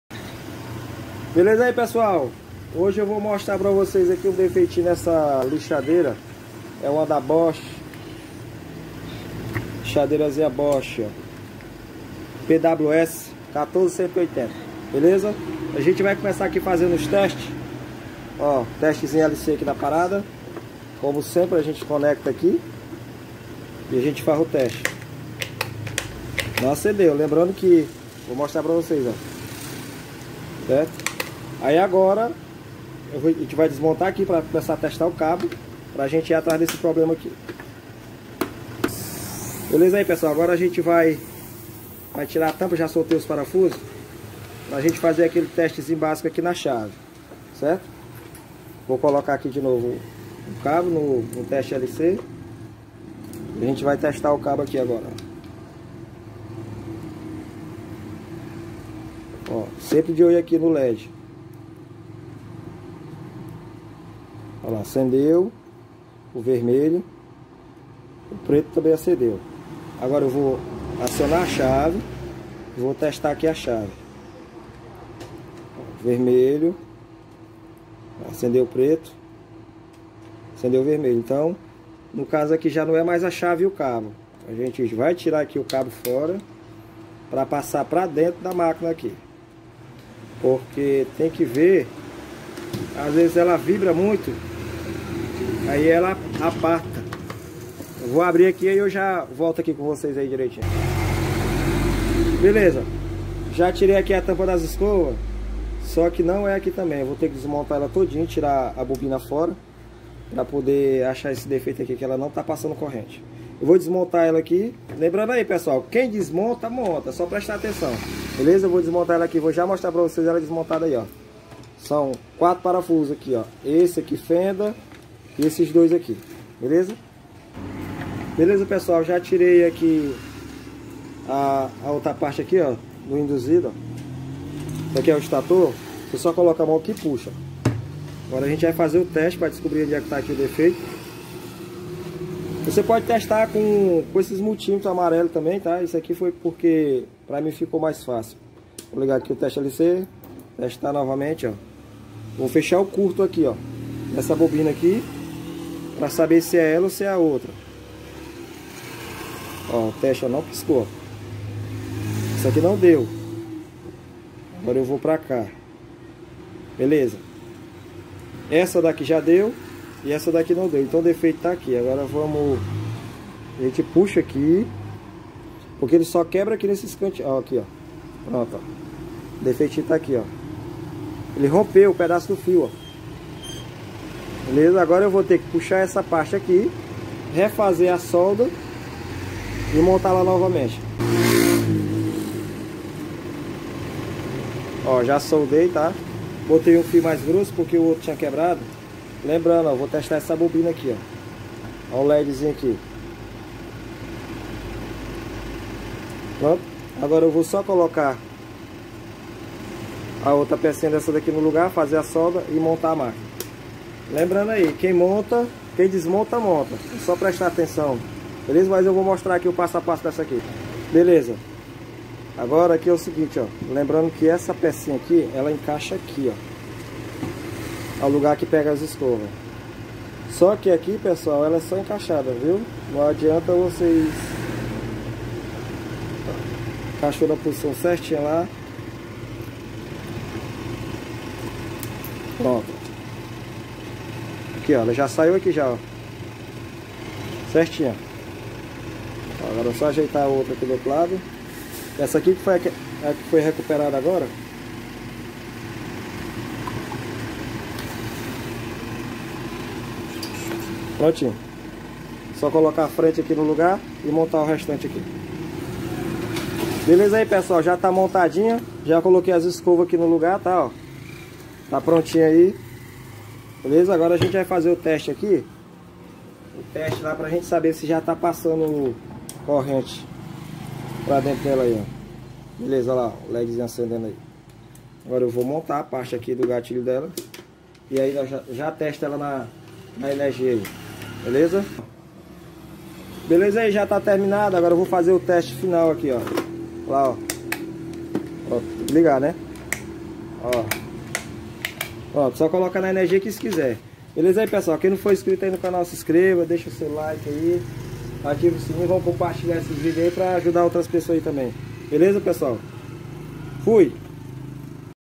Como conserta uma lixadeira
Lixadeira com estator com defeito